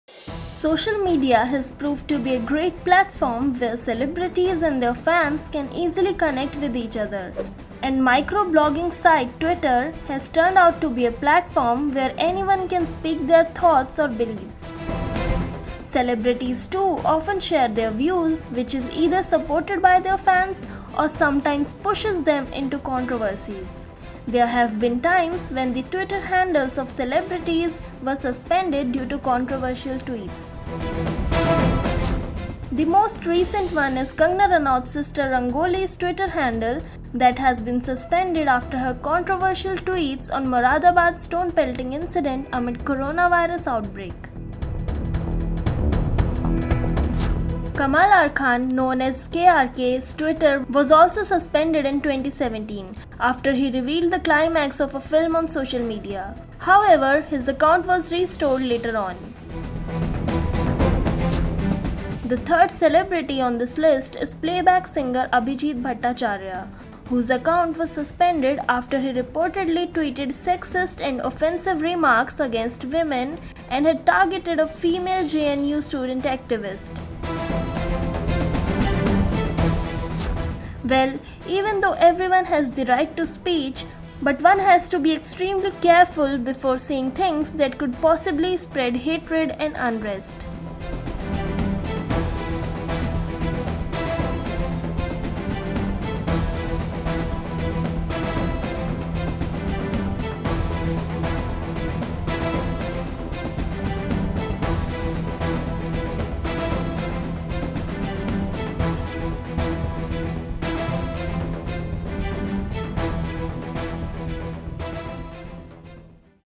Celebs Whose Twitter Account Was Suspended
Twitter is a great platform for celebrities to connect with their fans and share their opinions. However, many a times, celebrities find themselves in trouble after sharing their views on Twitter. Check out the list of Bollywood celebs whose Twitter account was suspended after posting controversial tweets.